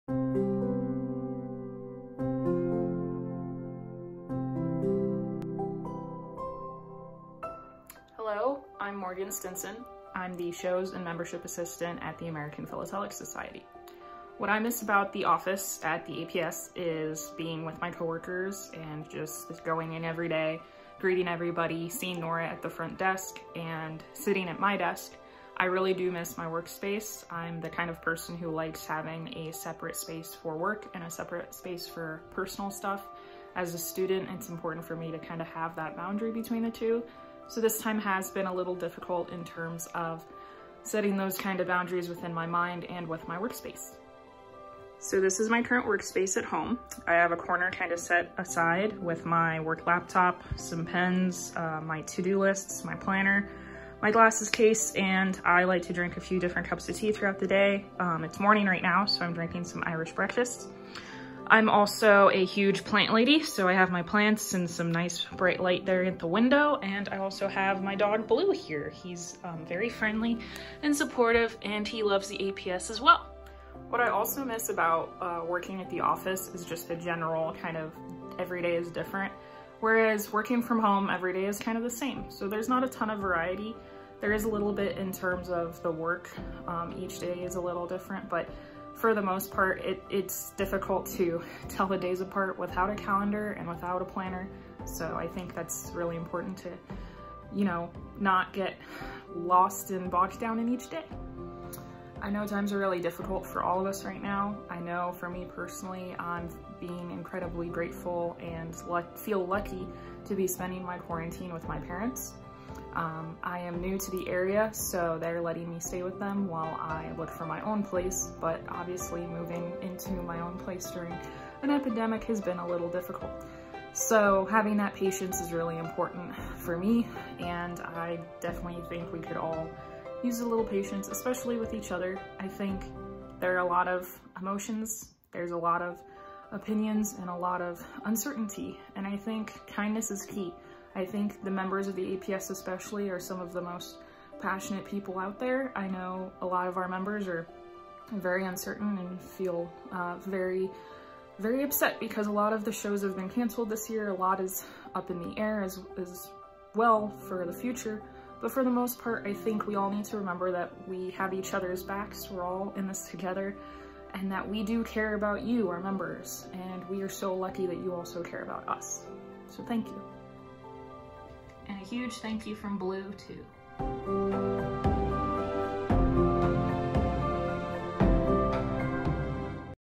APS Cares Morgan Stinson, APS Membership & Shows Assistant
APS Cares is a spotlight series showcasing APS Staff.  Meet Morgan Stinson, membership and shows assistant by day; roller derby player by nights and weekends...student and veteran too.  Morgan shares her care and appreciation for APS members and offers a real "day in the life of a remote worker during the shut-down" look into her life.